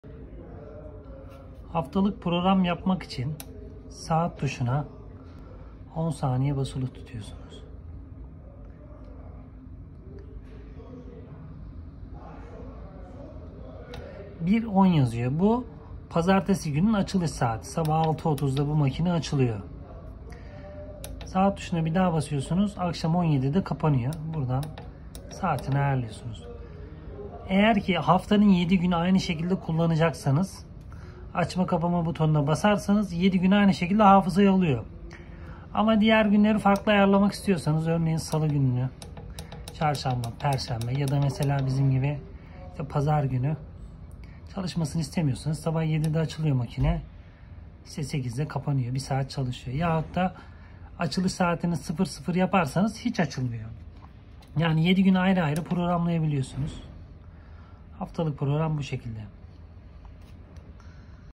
Ateşe akıllı çay kazanı Smart haftalık açılma kapanma programı nasıl yapılır
Ateşe akıllı çay kazanı haftalık açılış kapanış saatlerini ayarlama